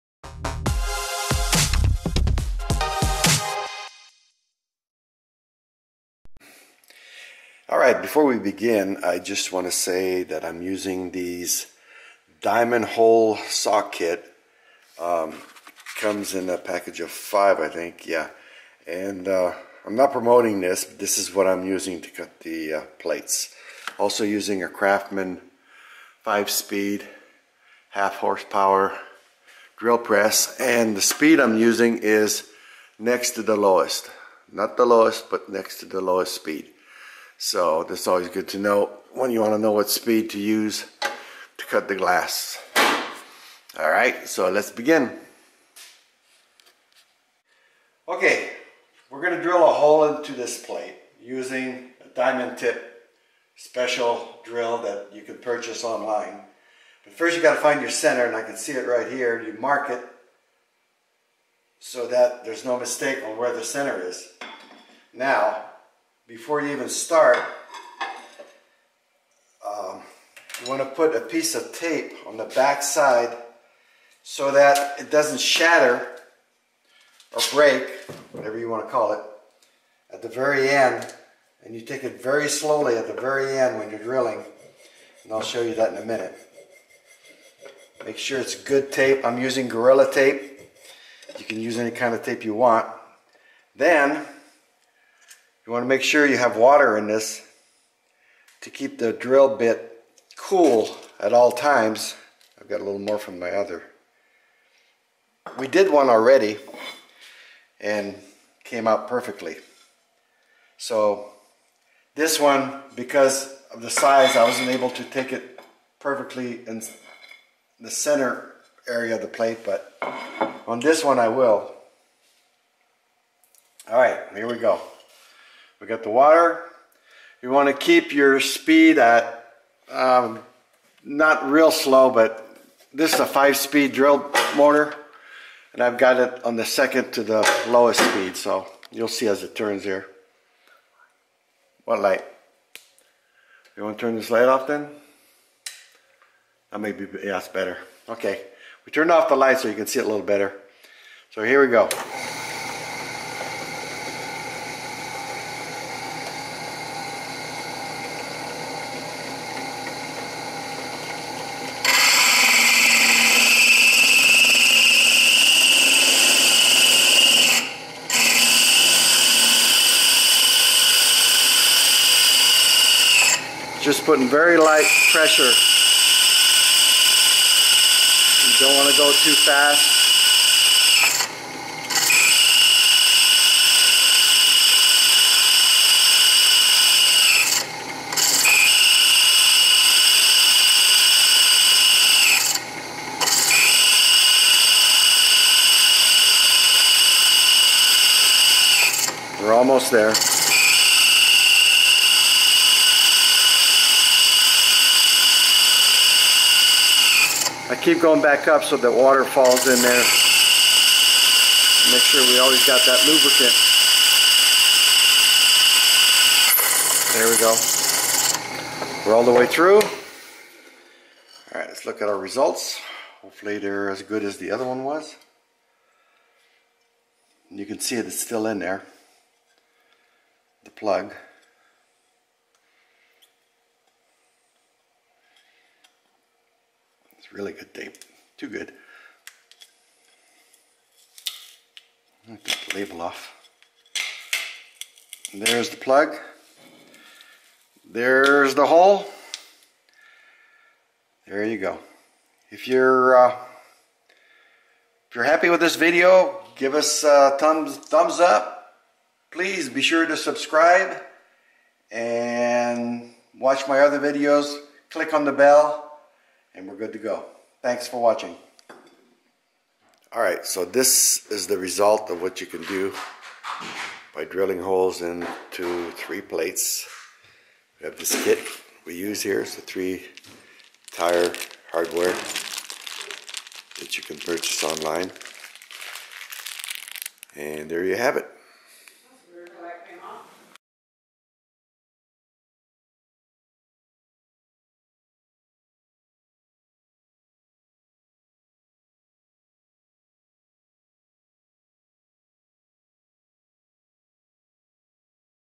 (300) How to drill a hole in a plate. 3 Tier hardware for 3 plates
How to safely drill holes in plates using a diamond hole set. Watch to the end, shows final results.